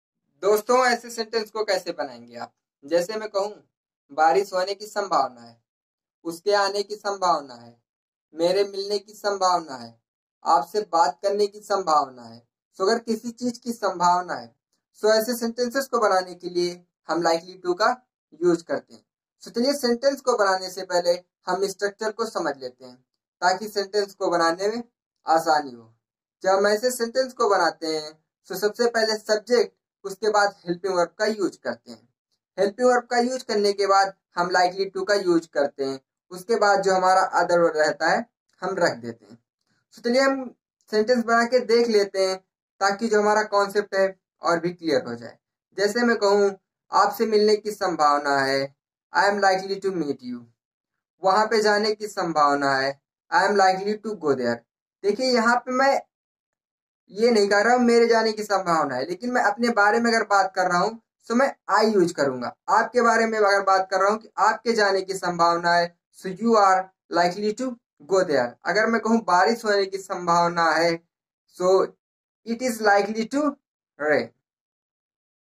Useoflikely dailyusesentences advanceenglishstructure grammar sentence shorts translation
Today I will teach you how to use likely to in English grammar